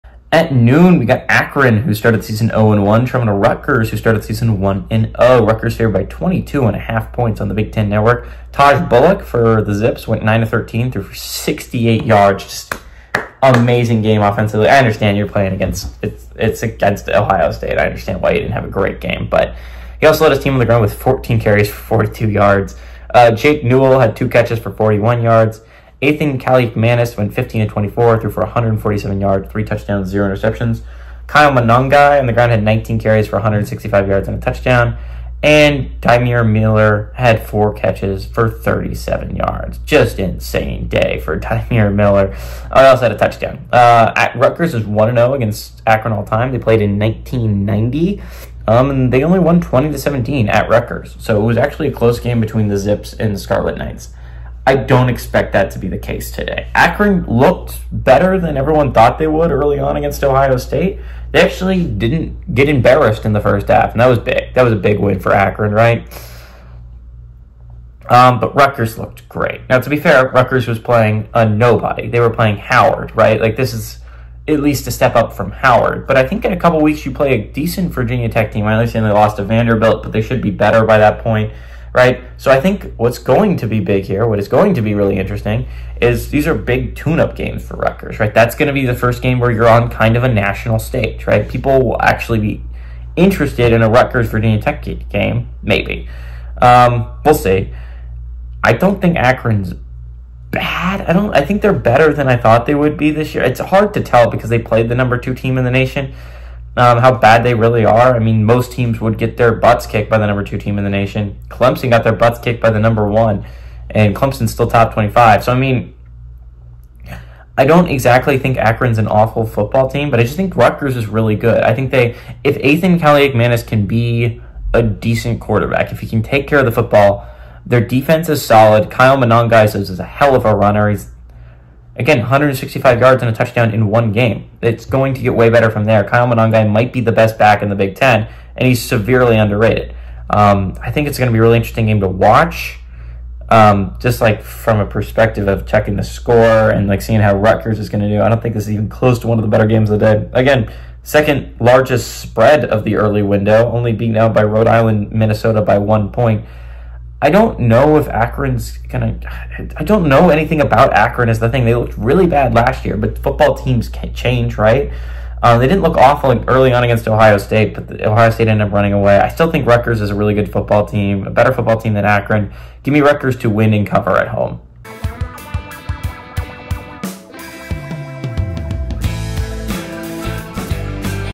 Akron @ Rutgers
In this video, GGB predicts one of the biggest games of the week. Will the Zips pick up the massive upset or will the Scarlet Knights stay undefeated? The only way to know is to watch the video.